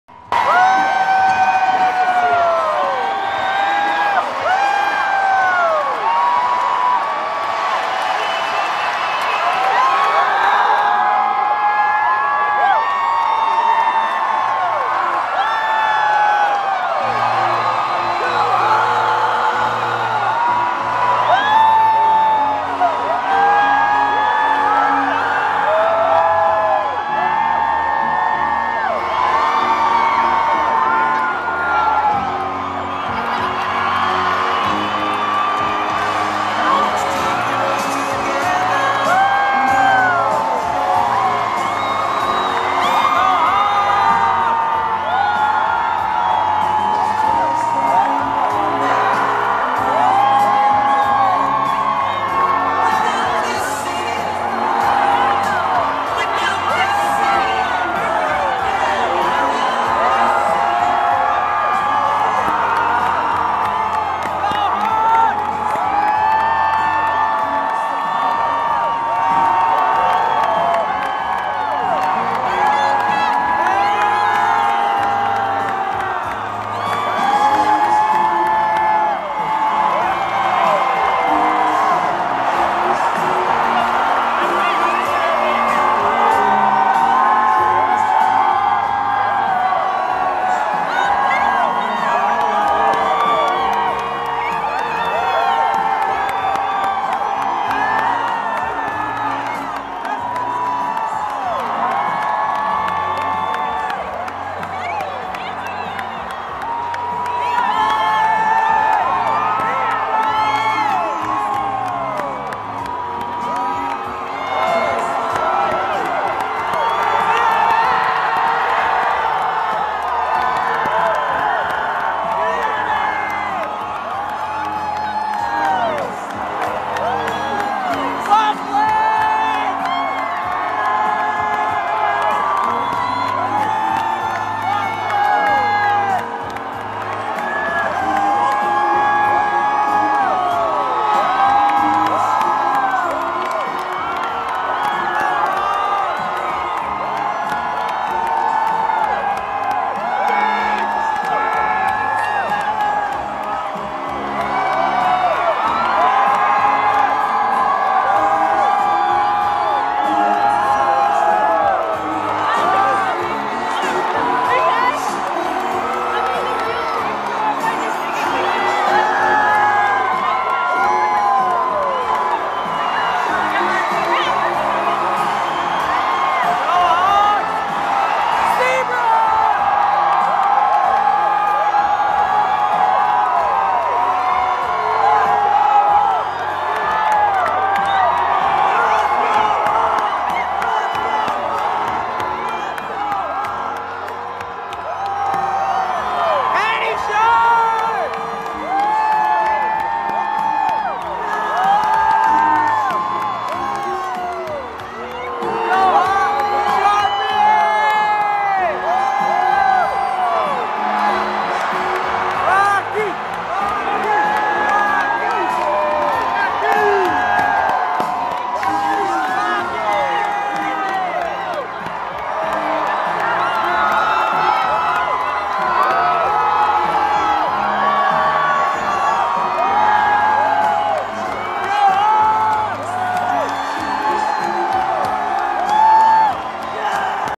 Hawks Rally
Carson, me and two million of my closest friends welcome home the Blackhawks and the Stanely CUP!  WELL DONE BOYS!